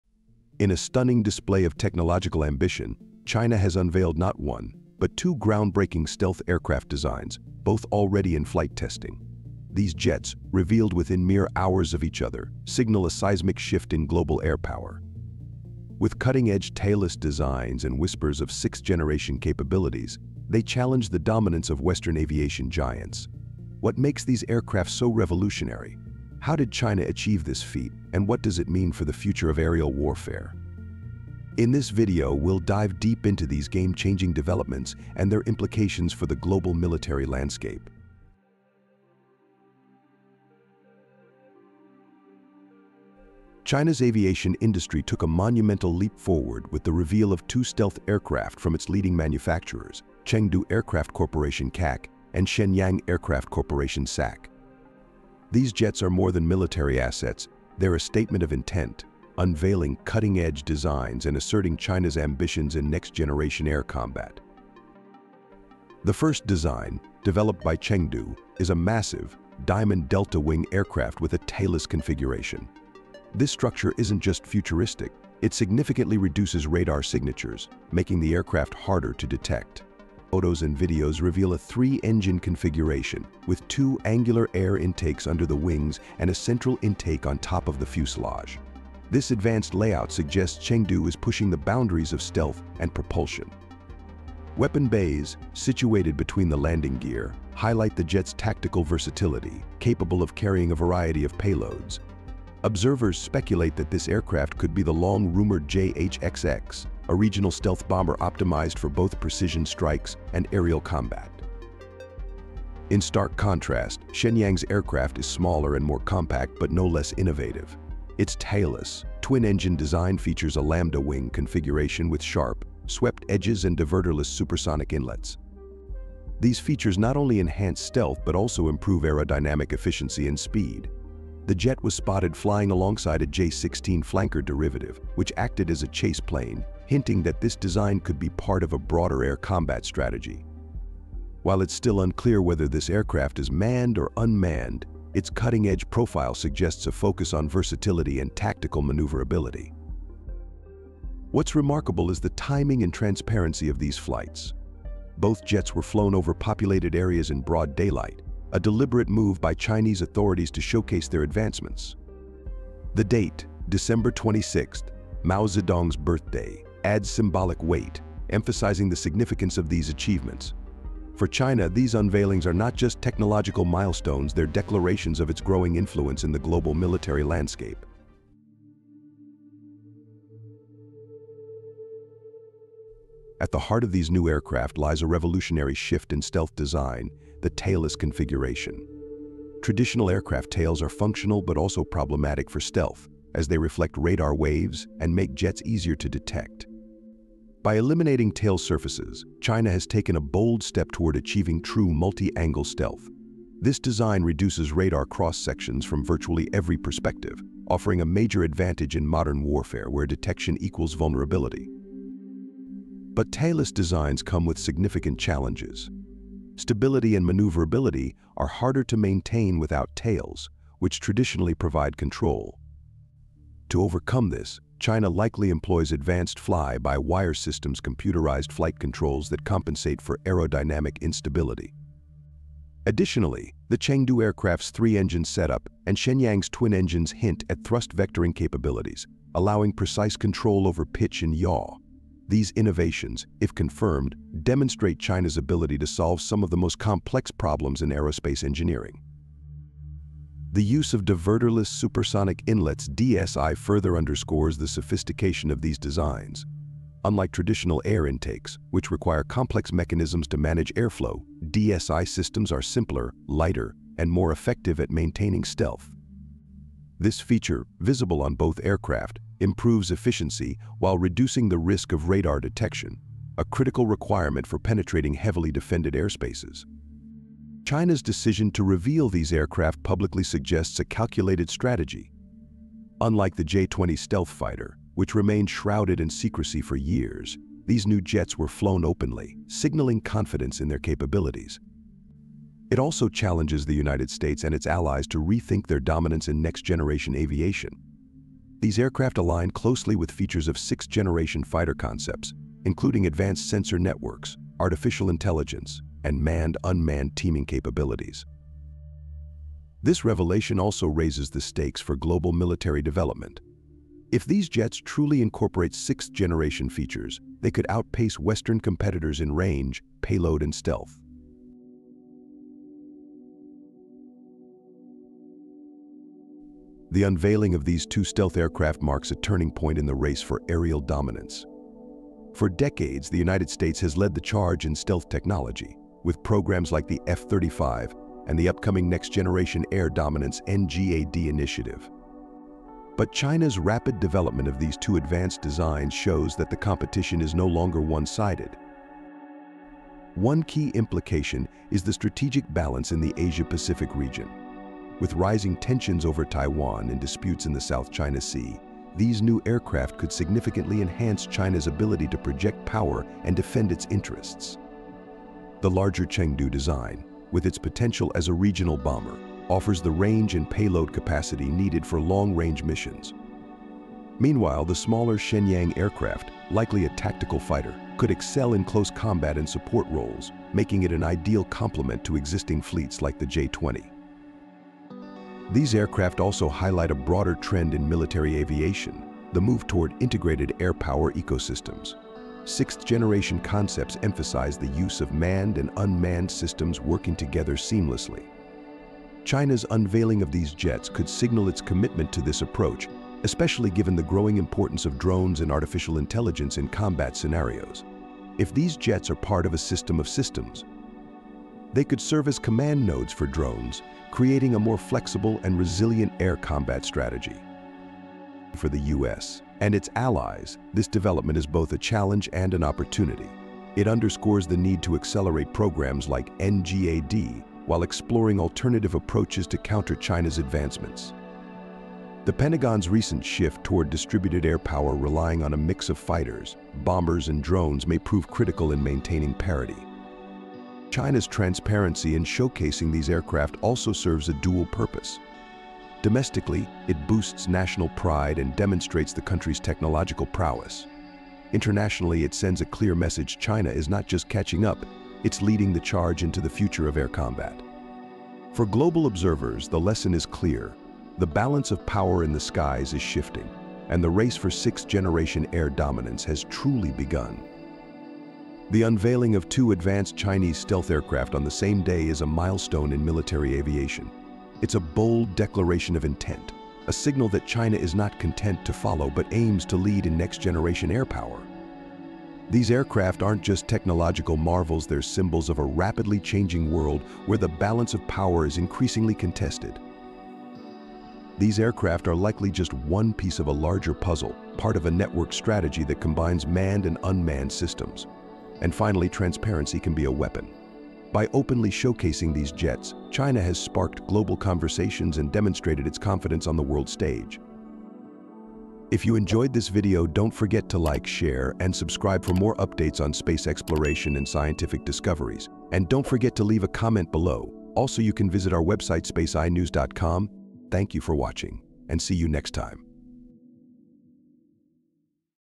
Stunning: China Unveils Another Sixth-Generation Stealth Jet Ahead of the USA!
China has just unveiled two groundbreaking stealth aircraft, redefining the future of air combat! One is a heavy diamond-delta wing bomber likely to be the JH-XX, while the other is a sleek, lambda-wing tactical fighter. Both feature tailless designs for ultimate stealth and incorporate advanced tech like thrust vectoring and AI integration. These revolutionary jets challenge global air power dynamics and signal China’s rise in sixth-generation aviation. Want to know how these designs could change the future of warfare? Watch now for all the details on this aviation breakthrough!